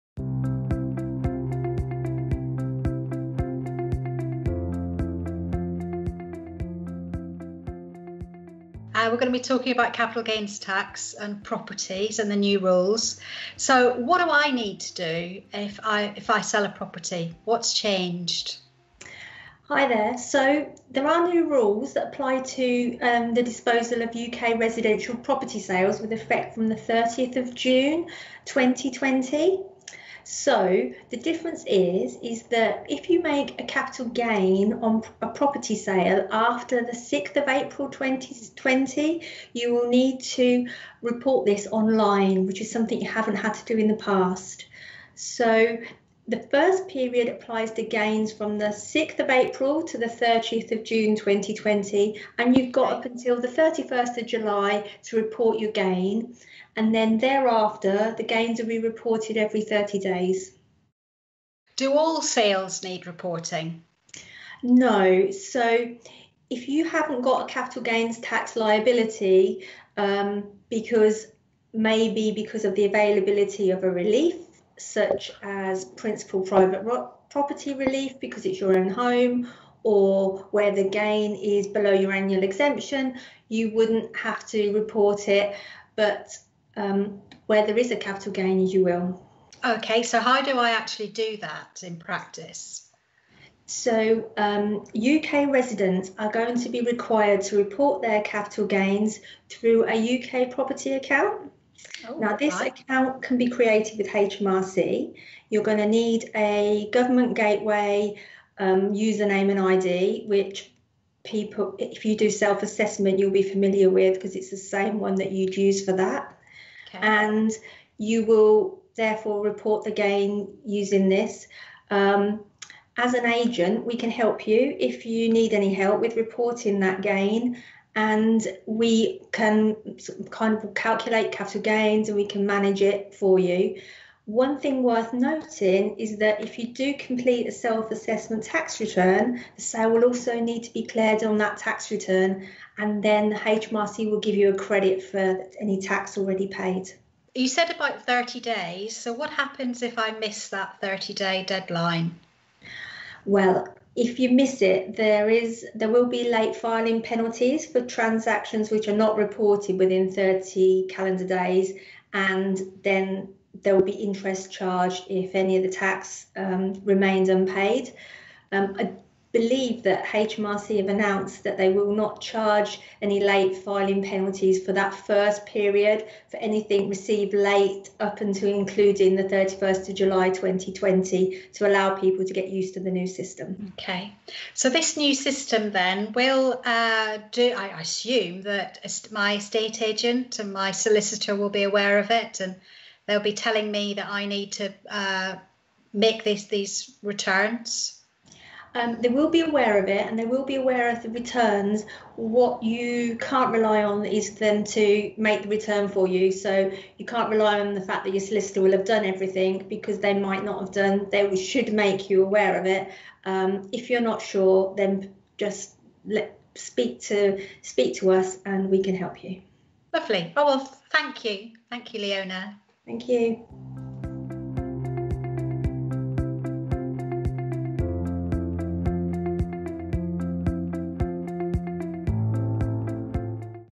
Elsby & Co - FAQ Series - Capital Gains Tax and Property
There are new rules surrounding how you report and pay capital gains tax on property sales from 6th April 2020. Find out more from our short video presented by Clare Elsby and Leona Bateman of Elsby & Co, Accountants & Business Advisors in Northampton.